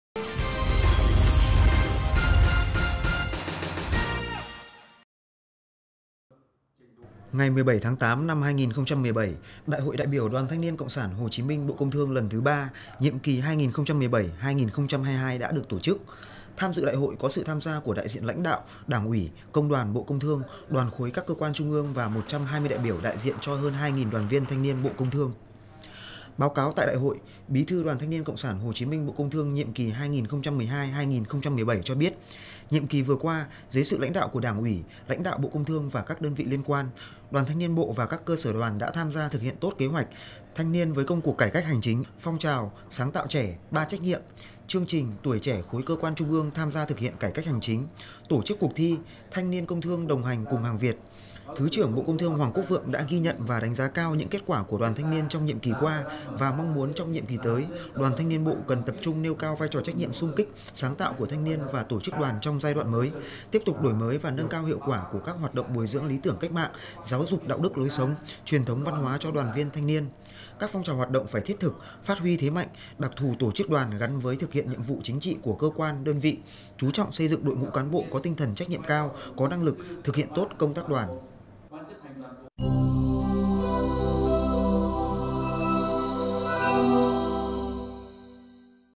Đại hội Đoàn TNCS HCM Bộ Công Thương nhiệm kỳ 2017 - 2022
Ngày 17/8/2017, Đại hội đại biểu Đoàn Thanh niên cộng sản Hồ Chí Minh Bộ Công Thương lần thứ III, nhiệm kỳ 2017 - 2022 đã được tổ chức. Tham dự Đại hội có sự tham gia của đại diện Lãnh đạo, Đảng ủy, Công đoàn Bộ Công Thương Đoàn Khối các cơ quan Trung ương và 120 đại biểu đại diện cho hơn 2000 đoàn viên thanh niên Bộ Công Thương...